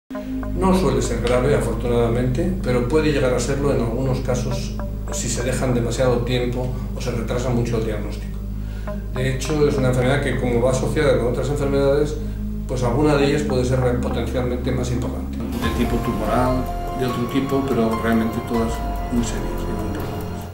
La CELIAQUIA es GRAVE ❓ Enfermedad CELIACA ➤ Dr Luis Rodrigo Sáez ESPECIALISTA en APARATO DIGESTIVO
Podría tener complicaciones 💥 MUY GRAVES 💥 en determinados casos. Te explicamos cuándo y por qué, de boca de uno de los mayores expertos mundiales enEnfermedad Celíaca y Sensibilidad al Gluten No Celíaca, el Dr. Luis Rodrigo Sáez, en nuestra NUEVA SERIE DE VÍDEOS CORTOS.